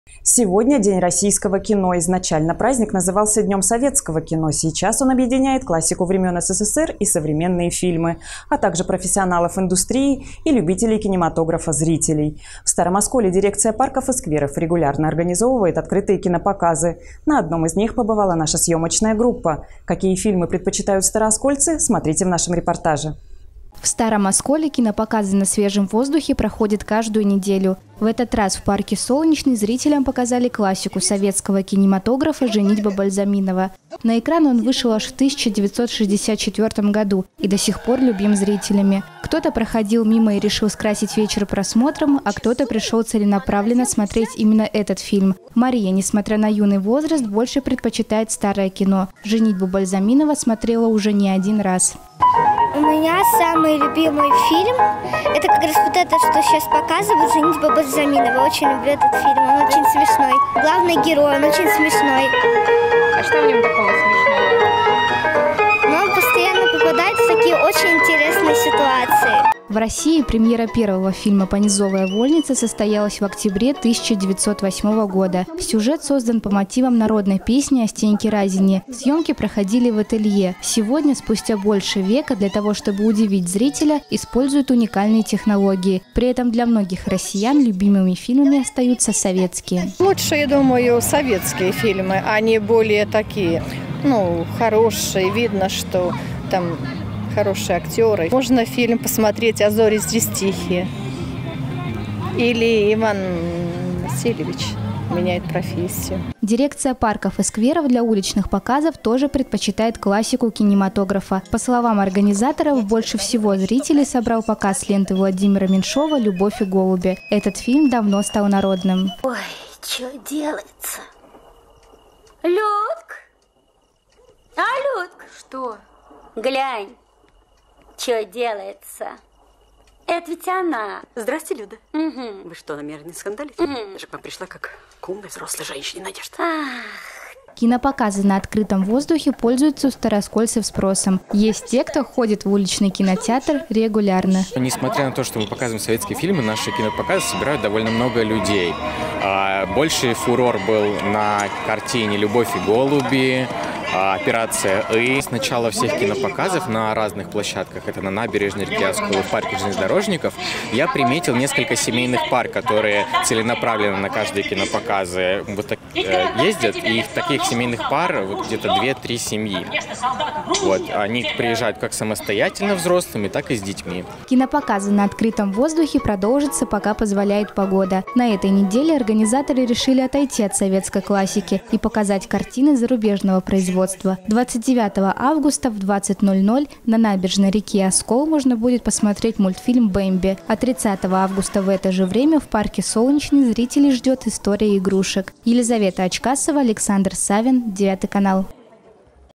"Кина... будет!": День российского кино в Старом Осколе отметили просмотром любимых комедий
Сегодня - День российского кино. Изначально праздник назывался Днем советского кино. Сейчас он объединяет классику времен СССР и современные фильмы, а также профессионалов индустрии и любителей кинематографа - зрителей. В Старом Осколе дирекция парков и скверов регулярно организовывает открытые кинопоказы, на одном из них побывала наша съёмочная группа. Какие фильмы предпочитают старооскольцы, смотрите в нашем репортаже.